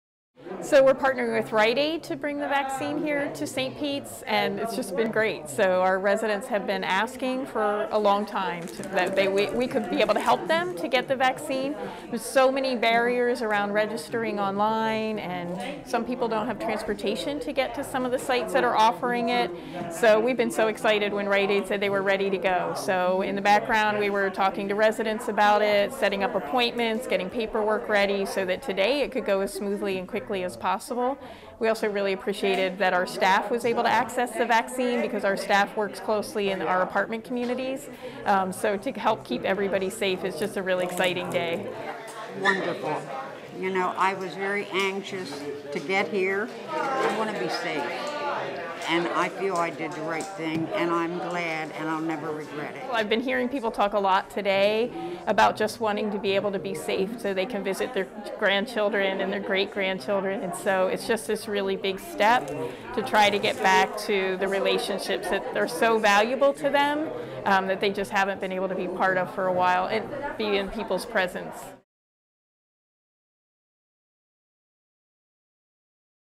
Bringing the Covid-19 vaccine to Columbia residents
Residents in Columbia were able to get their first dose of the Covid-19 vaccine without having to leave their apartment complex.

HDC MidAtlantic is partnered with RiteAid Pharmacy for a COVID-19 vaccination clinic at Trinity House Apartments and St. Peter Apartments in Columbia. The clinic was for residents of the two senior housing communities, and for staff.